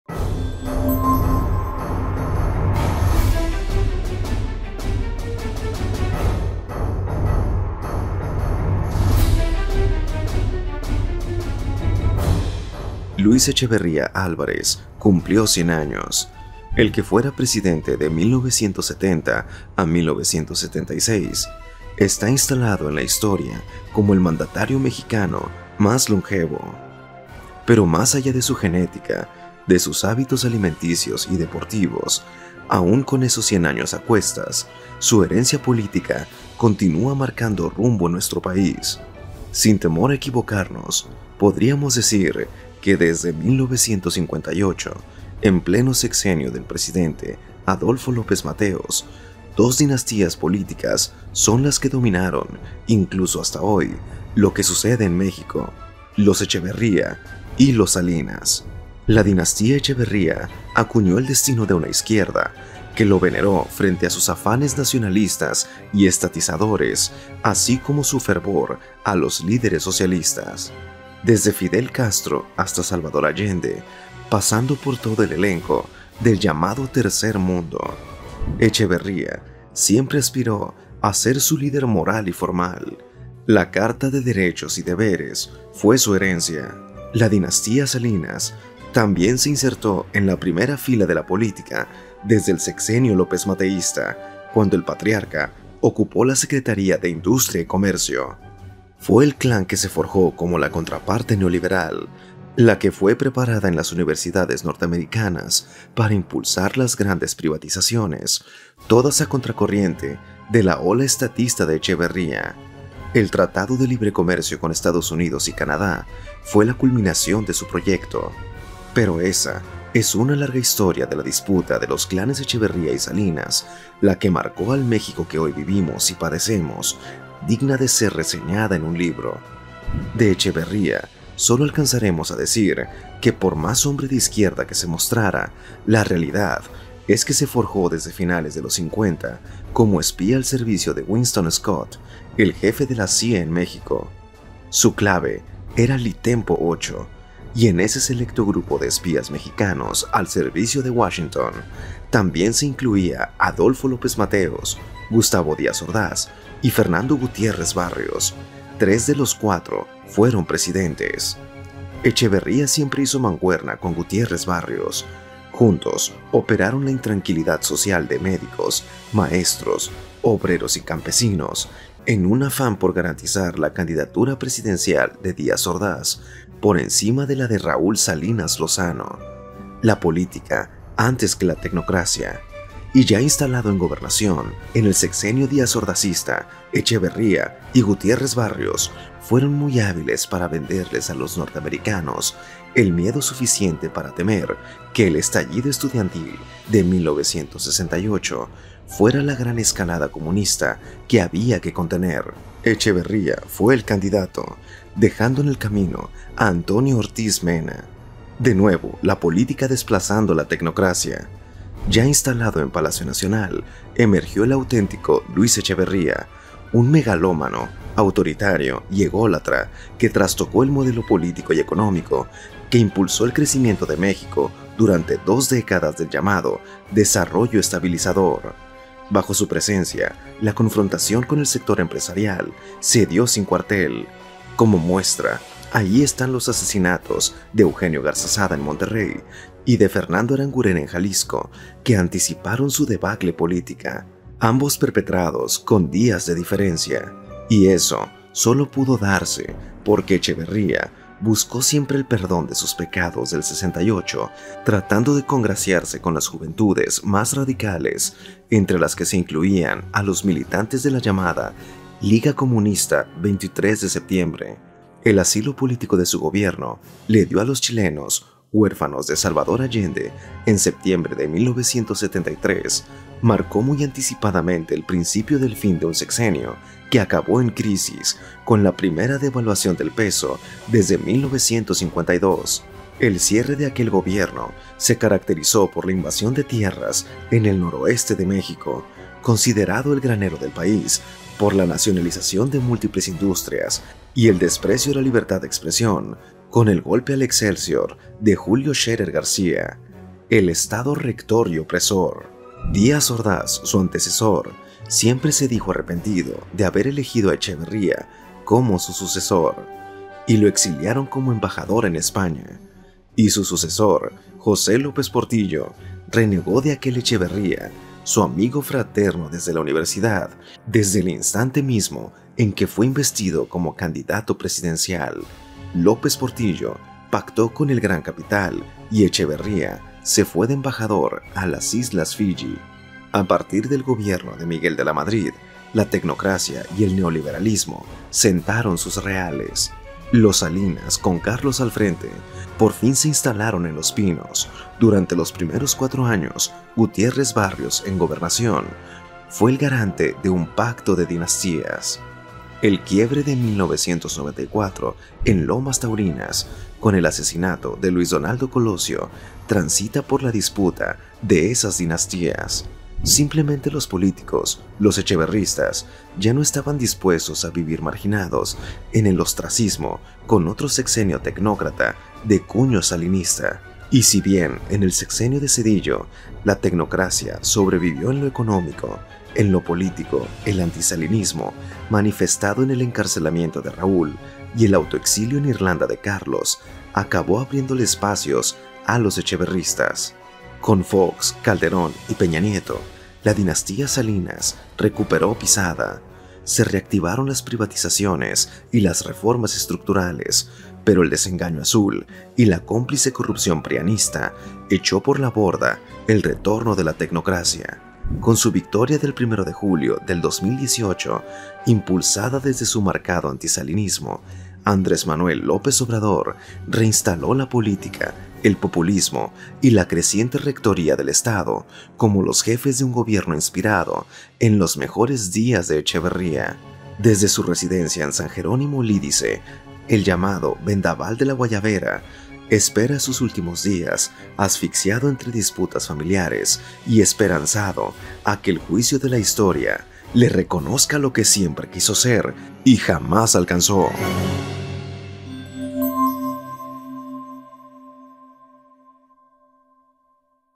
Echeverría 100
Desde su residencia en San Jerónimo Lídice, Luis Echeverría Álvarez, el llamado Vendaval de la Guayabera espera sus últimos días, asfixiado entre disputas familiares y esperanzado a que el juicio de la historia le reconozca lo que siempre quiso ser y jamás alcanzó.

👉La opinión de Ramón Alberto Garza en QueAlguienMeExplique